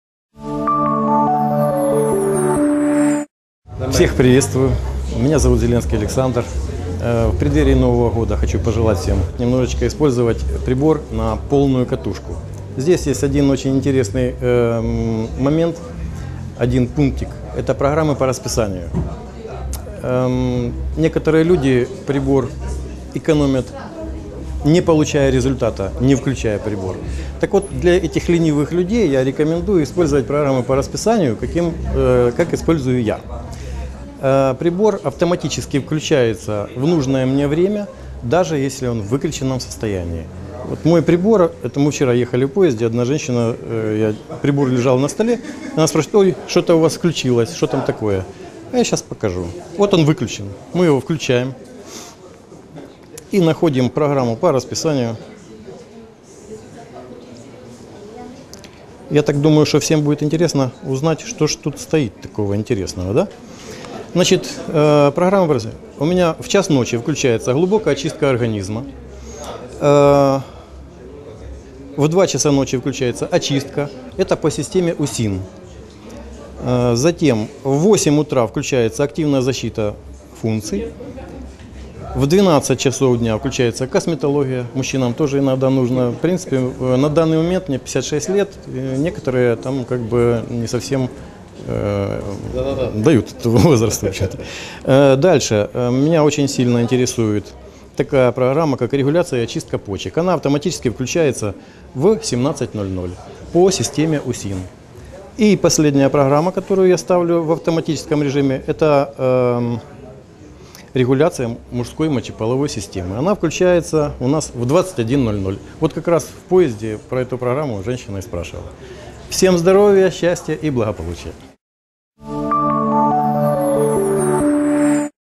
✔ Преимущество использования приборов Smart Life и возможности запуска программ по расписанию. Отзыв
Система сопровождения клиентов Smarti System.
Официальный сайт системы сопровождения Smarti System.

✔ Преимущество использования приборов Smart Life и возможности запуска программ по расписанию. Отзыв Александр Зеленский